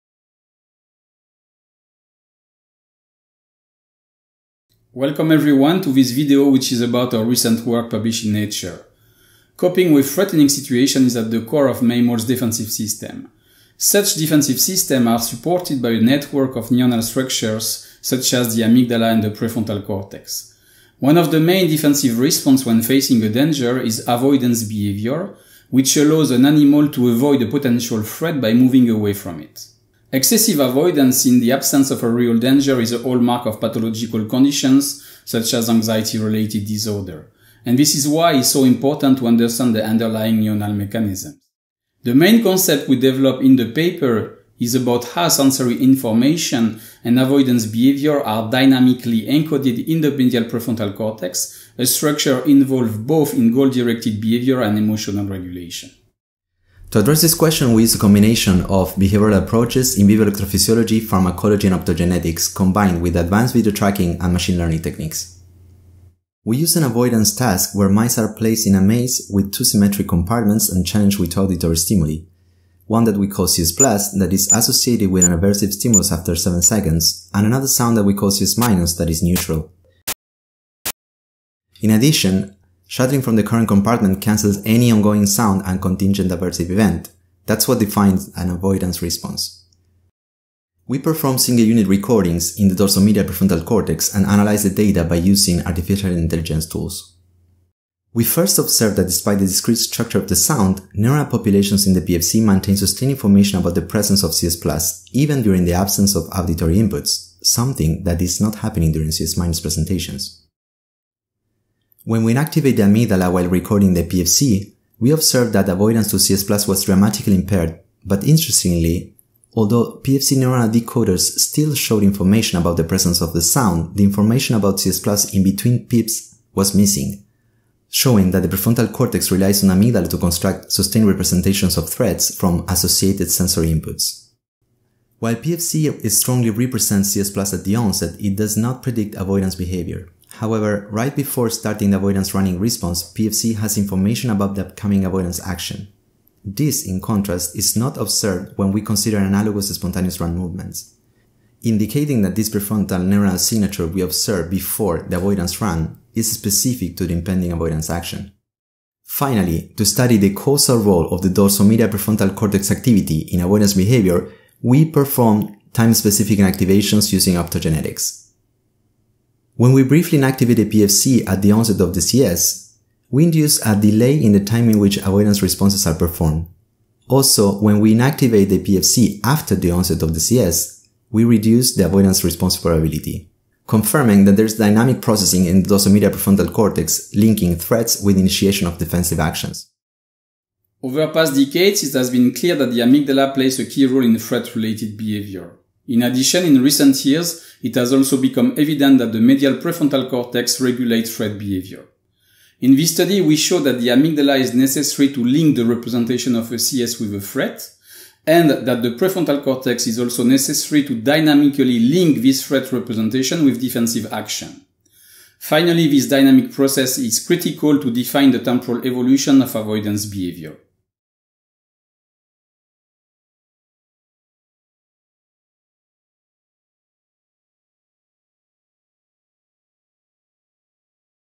Dynamical prefrontal population coding during defensive behaviours
Nature 2021.

Daniel Jercog, Nanci Winke, Kibong Sung, Mario Martin Fernandez, Claire Francioni, Domitille Rajot, Julien Courtin, Fabrice Chaudun, Pablo E. Jercog, Stephane Valerio, Cyril Herry.

Dorsomedial prefrontal cortex represents threat and defensive action initiation, and the dynamic interplay between those representations dictates the temporal evolution of avoidance response to threats.